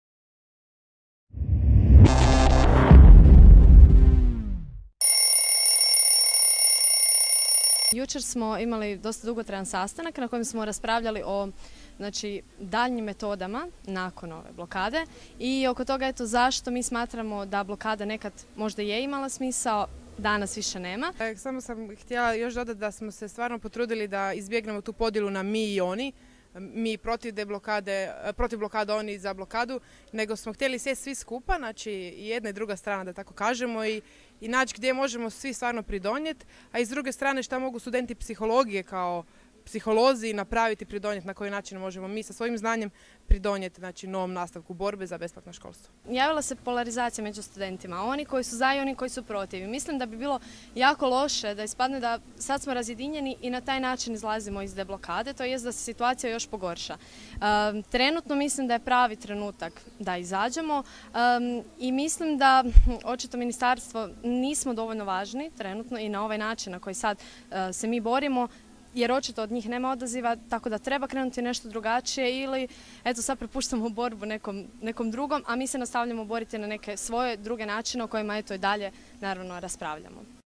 “Očito ministarstvu nismo dovoljno važni…” (studentice Odsjeka za psihologiju, 2009.)
Studenti Odsjeka psihologije Filozofskog fakulteta u Rijeci smatraju da je blokada nekada imala smisao, ali je došlo vrijeme za neke nove metode borbe. Ovdje poslušajte njihove argumente.